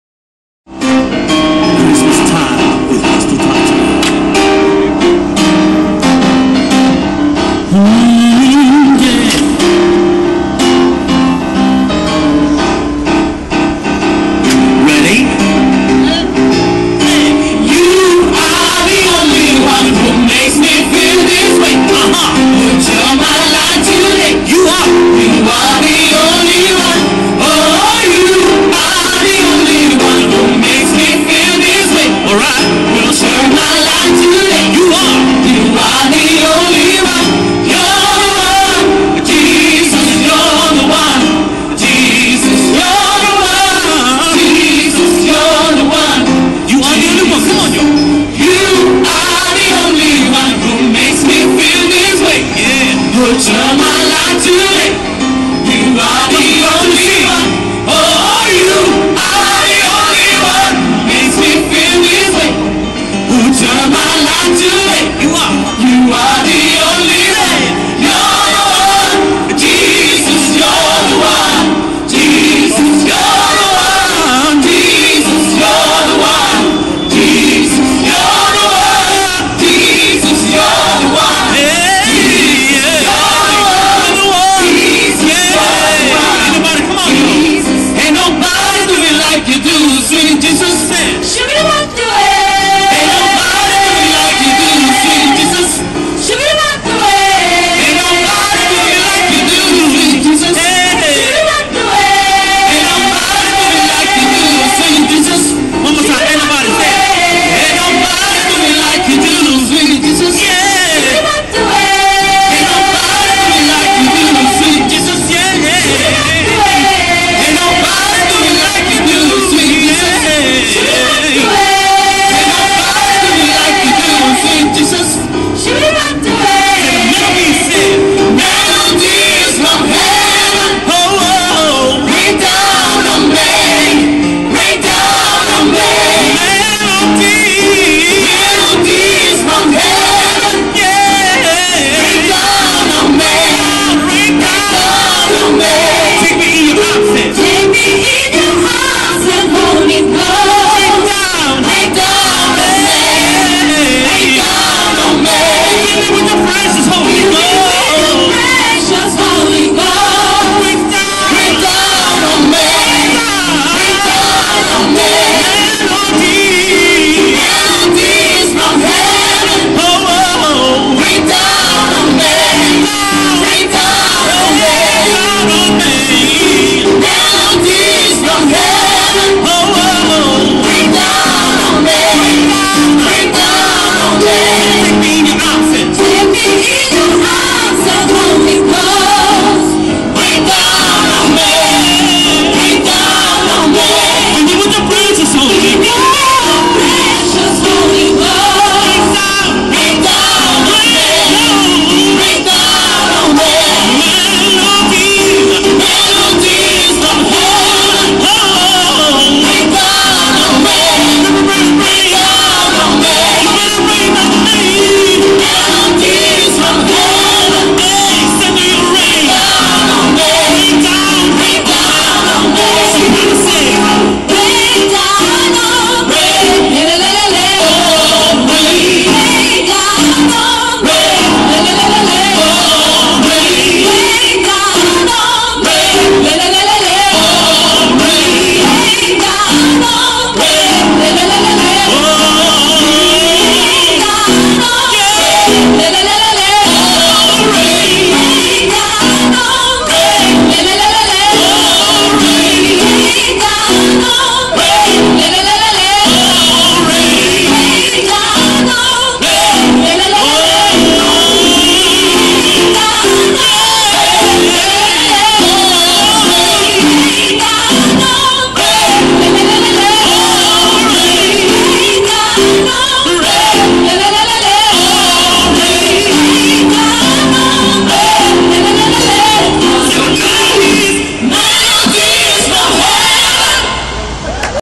Gospel Factory en Velazquez (Promo Citroen) (Parte 1)
Mini-concierto en directo de Gospel Factory en la calle Velazquez promocionando a Citroen